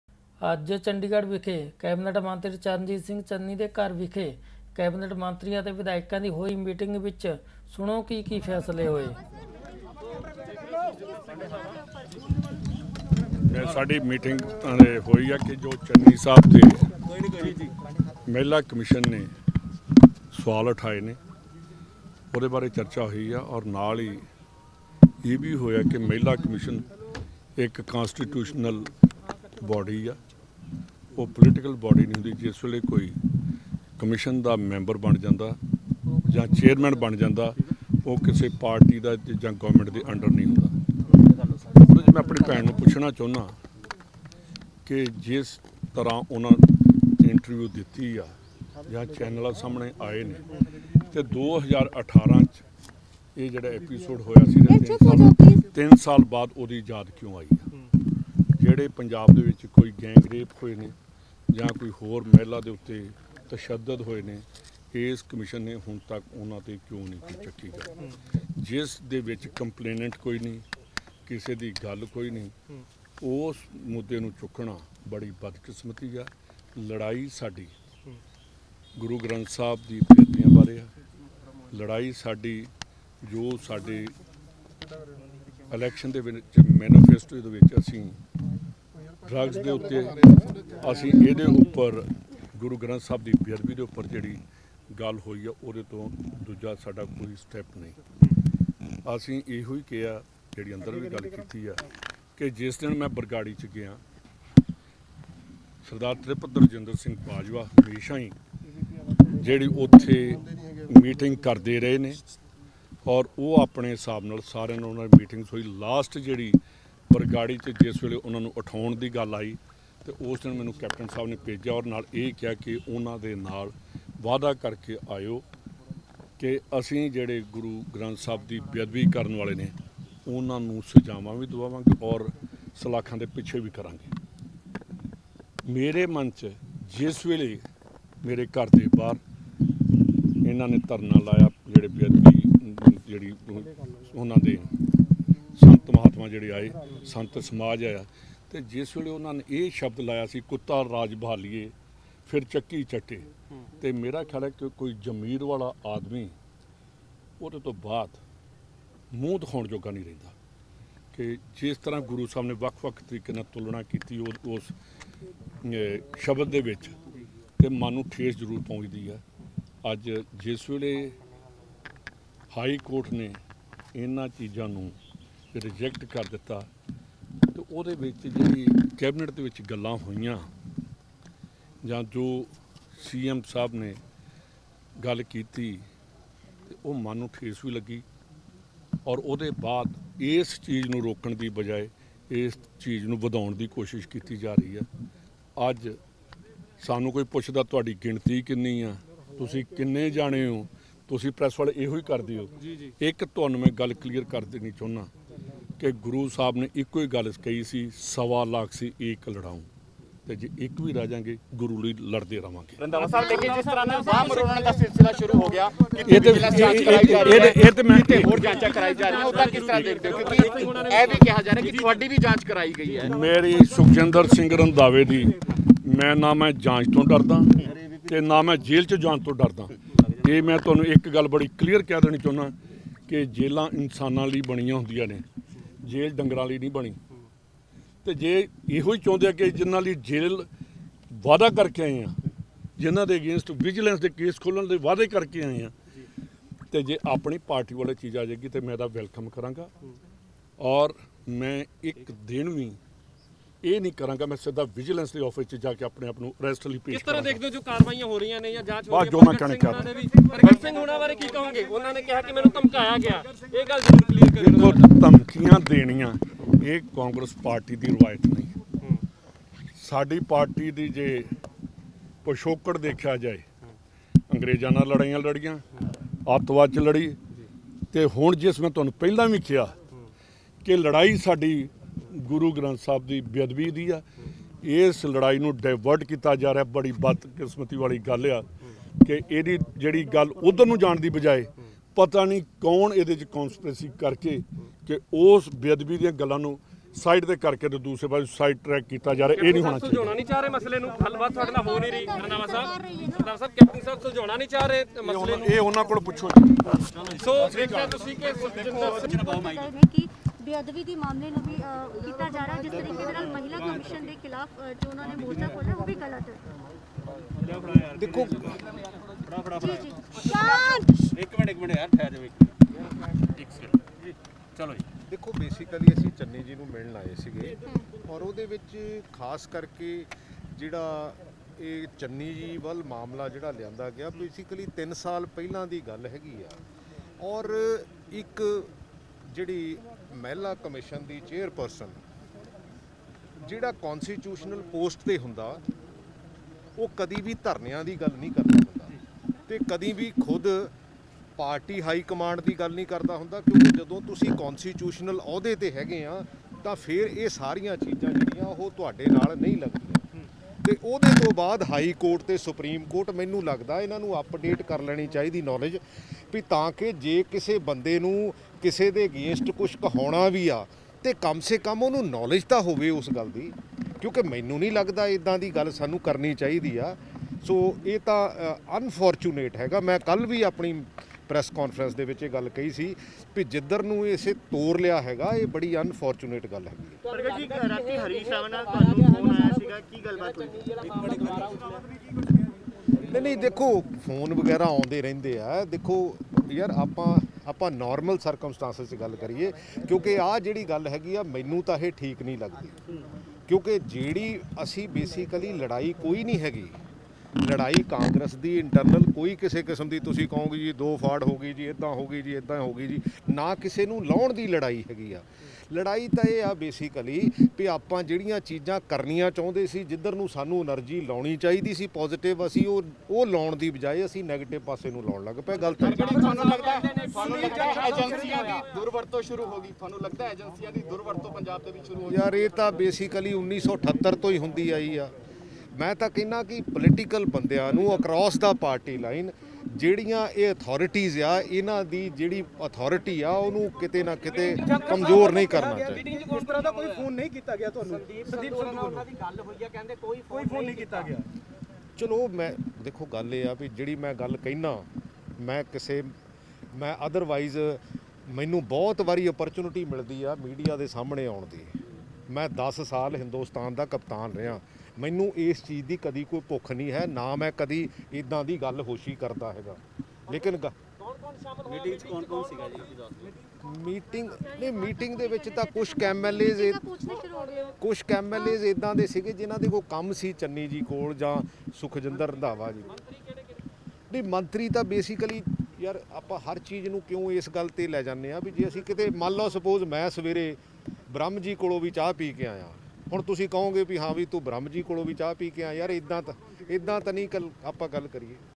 ਸ਼੍ਰੀ ਗੁਰੂ ਗ੍ਰੰਥ ਸਾਹਿਬ ਜੀ ਦੀ ਬੇਅਦਵੀ ਤੇ ਕੀ ਕਿਹਾ ,ਕੈਬਨਿਟ ਮੰਤਰੀ ਸੁਖਜਿੰਦਰ ਸਿੰਘ ਰੰਧਾਵਾ
Gur Kirpatv# Live Gurbani Recoding